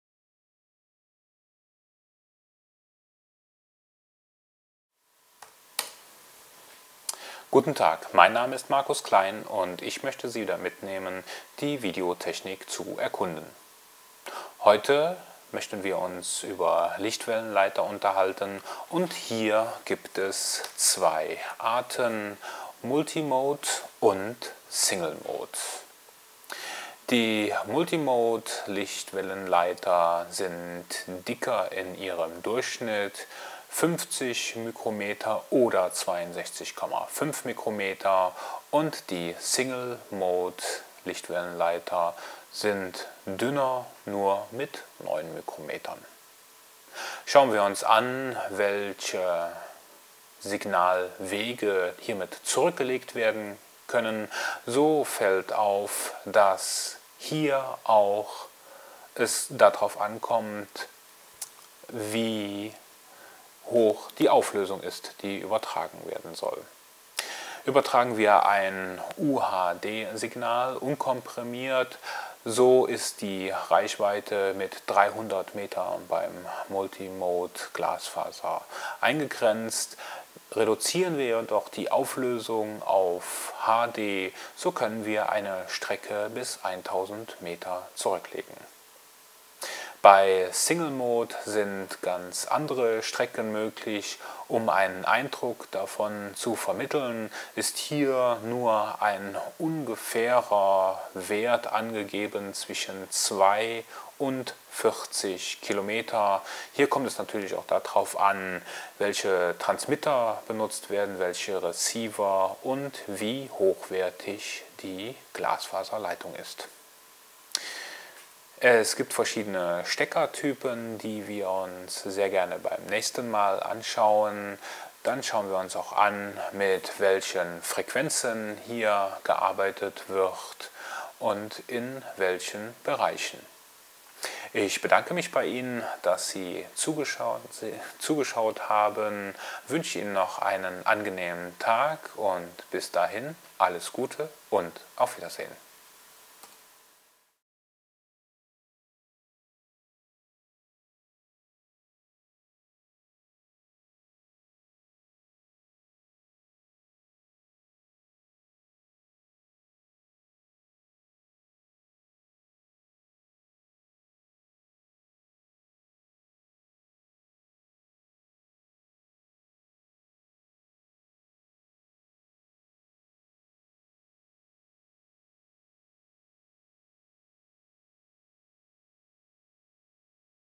VideoExpert.Engineer # 026 - Multi-Single-Mode-LWL
Die LWL Kabel unterscheiden sich zwischen Multi-Mode und Single-Mode. Hauptunterschied ist für den Videobereich die mögliche Übertragungslänge eines Signal.